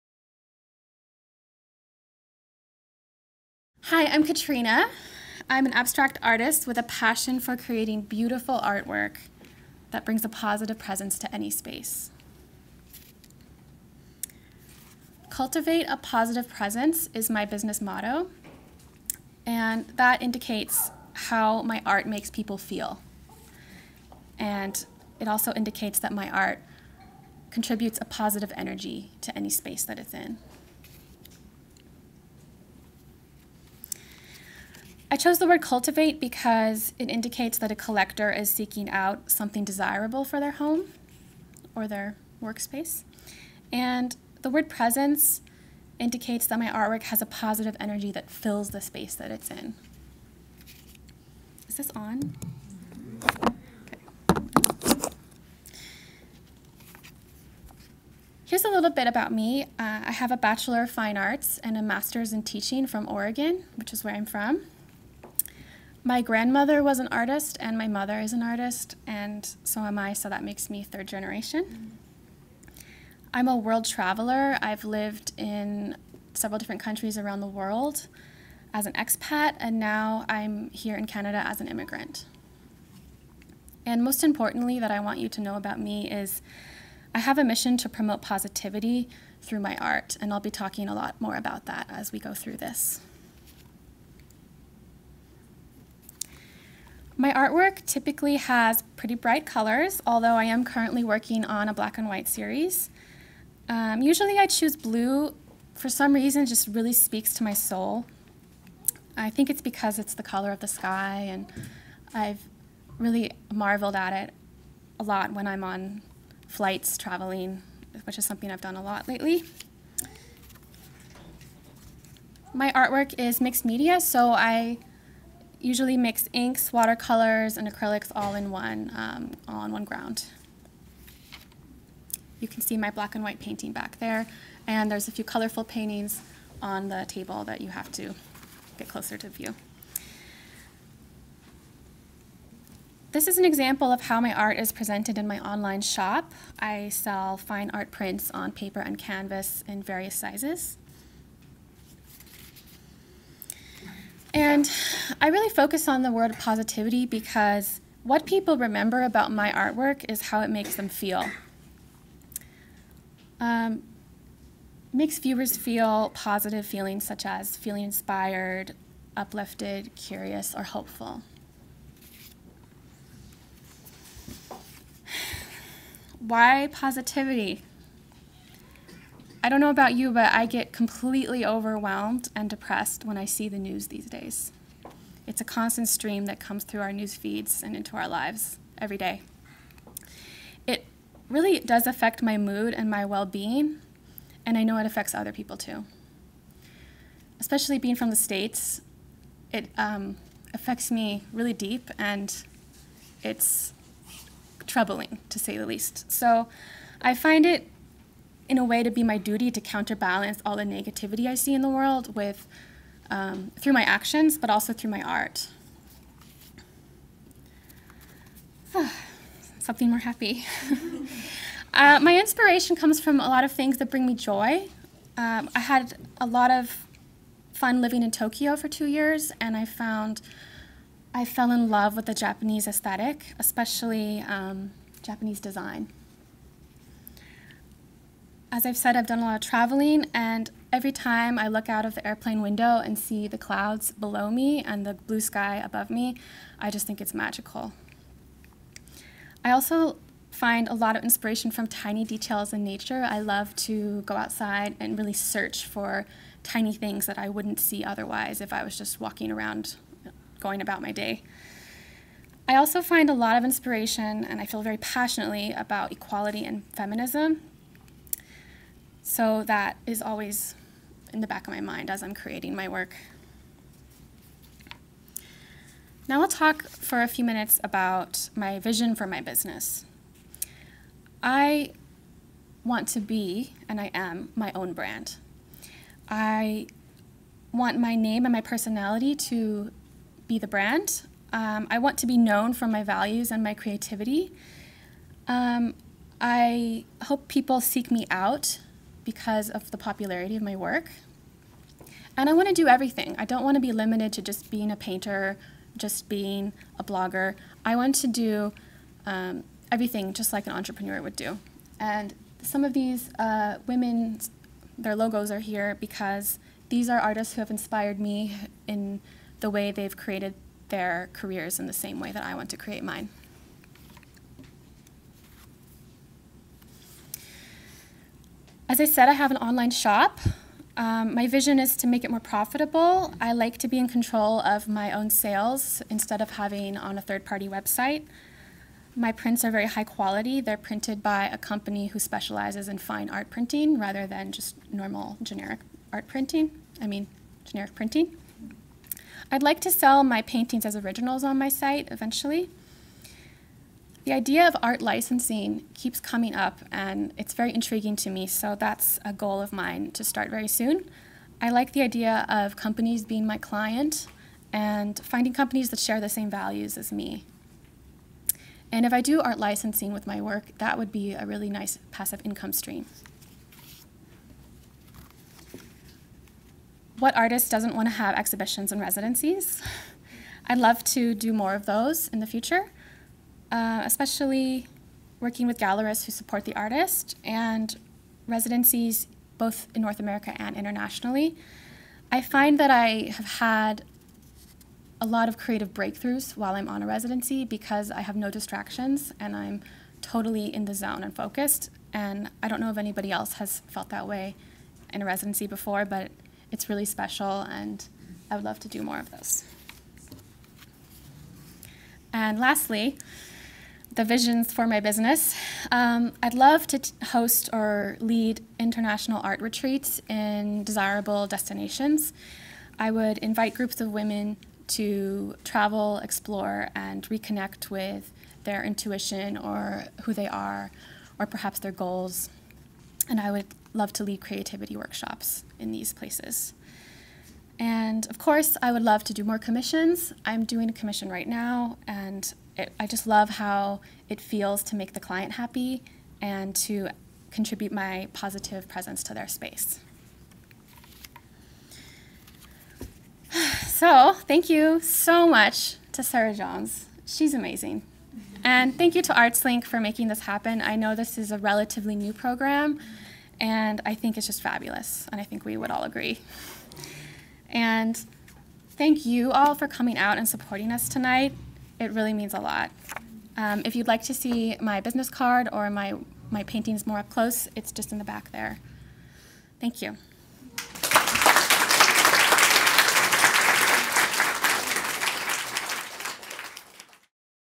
CATAPULT Pitch Day:Katrina Slade
Meet ArtsLink NB's latest graduates of the CATAPULT Arts Accelerator.

FRIDAY NOV 30, 2018 | 7 PM
Beaverbrook Art Gallery
Fredericton, NB

Katrina Slade, originally from Oregon, has an undefinable urge to create organically ethereal compositions through mixed media experiments. She works in layered combinations of acrylics, watercolours, and inks. Slade’s style is described as otherworldly, non-objective, and centred in the basic elements of colour and harmony. The abstract nature of her aesthetic invites the viewer to arrive at their own unique interpretation of her work.

Slade’s evolving style is influenced by her international lifestyle, especially from her time living in Tokyo, Japan, where she fell in love with the Japanese aesthetic. World travel has been instrumental in her artistic growth. Slade is currently living and working in Fredericton.